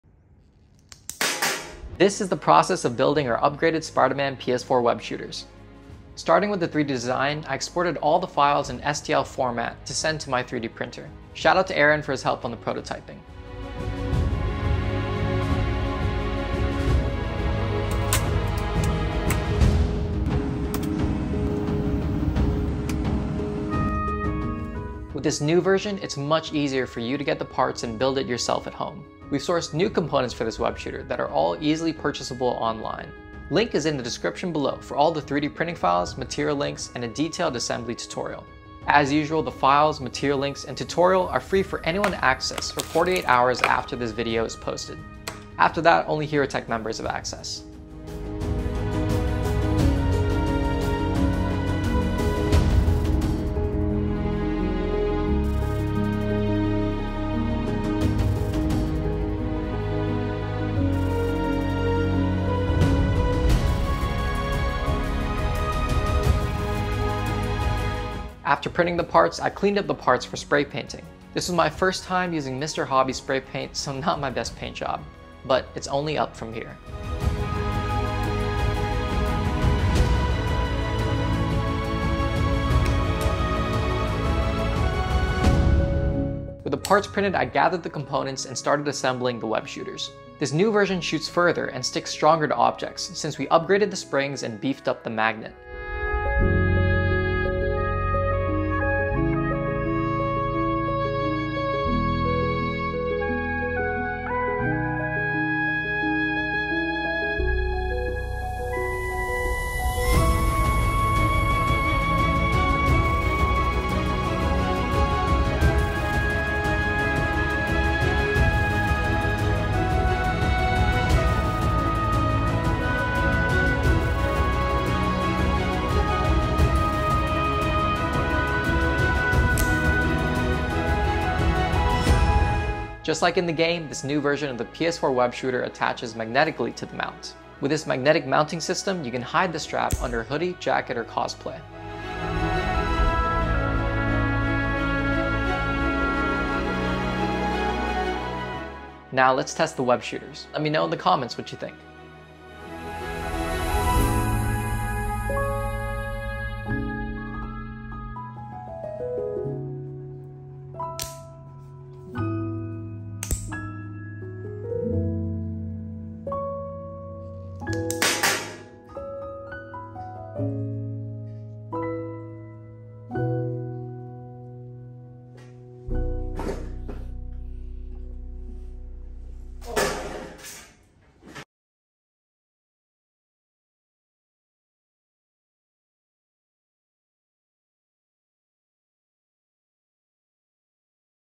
Making Upgraded Spider-Man PS4 Web Shooters (Functional)
Upgraded our functional Spider-Man PS4 web shooters so that you can easily make it yourself, it shoots further, sticks to more things, and magnetically mounts to your wrist. This video shows the process of making it!